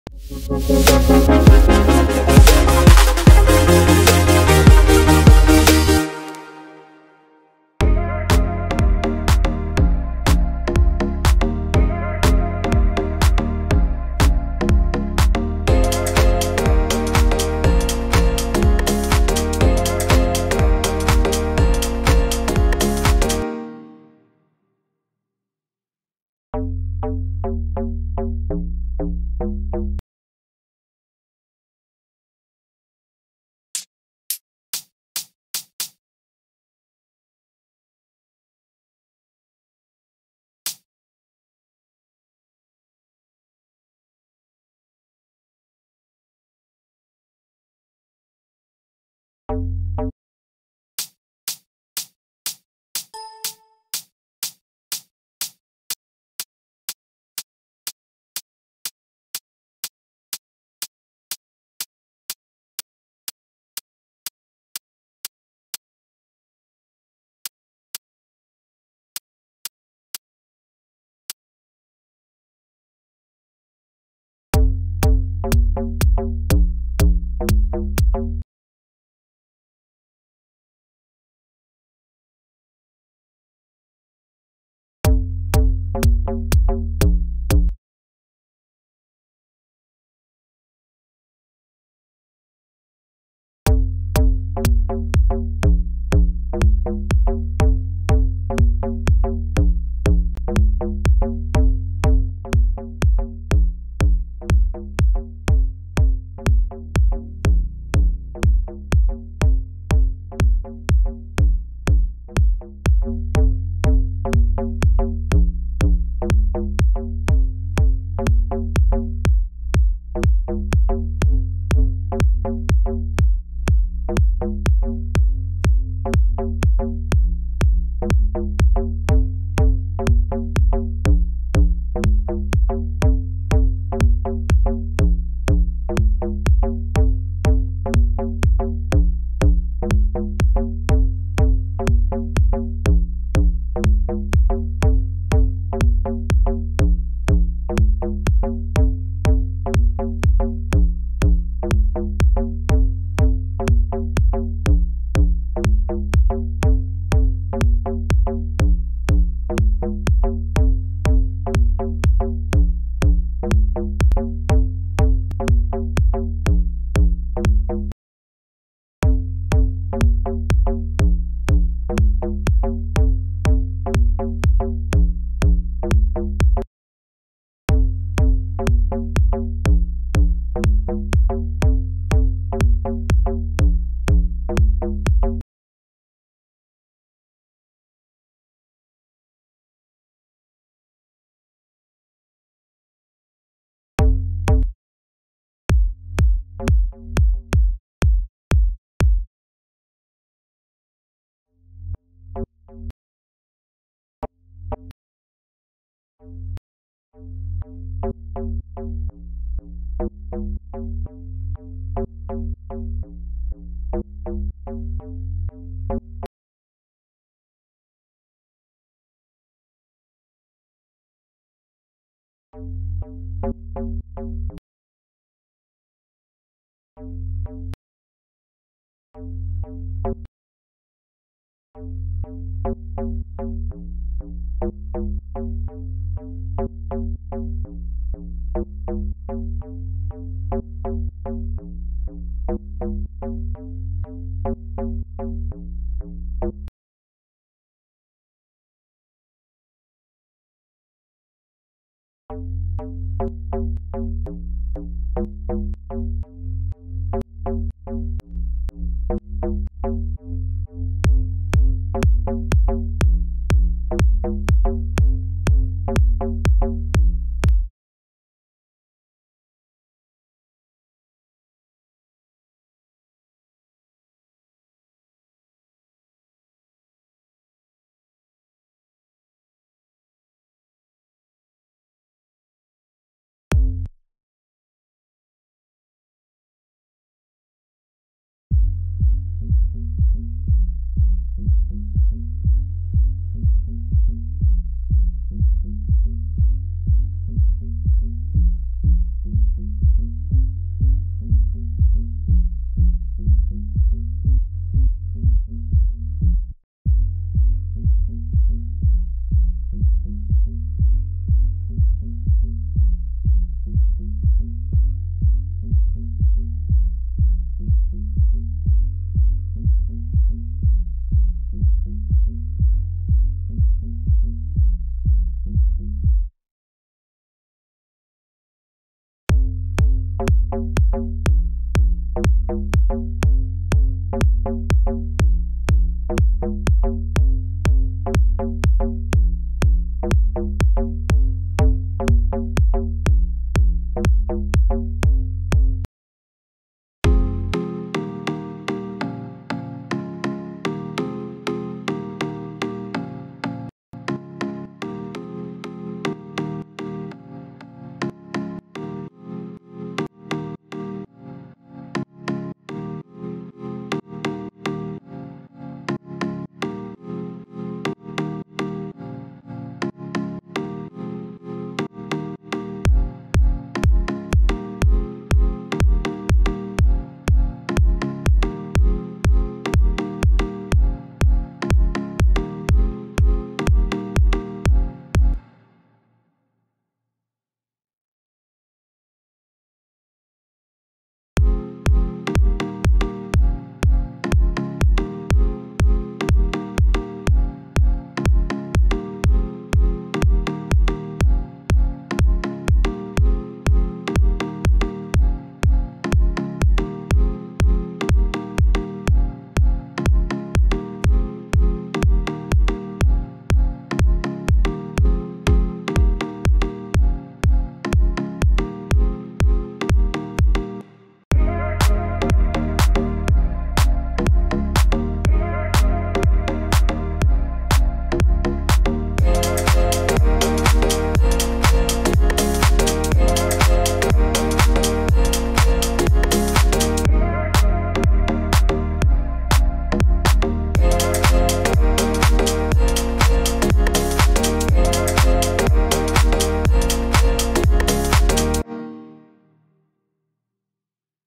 How To Sidechain (3 different methods)
Hi! In this video, I'll show you some sidechain techniques in FL Studio, i Hope you like it! :)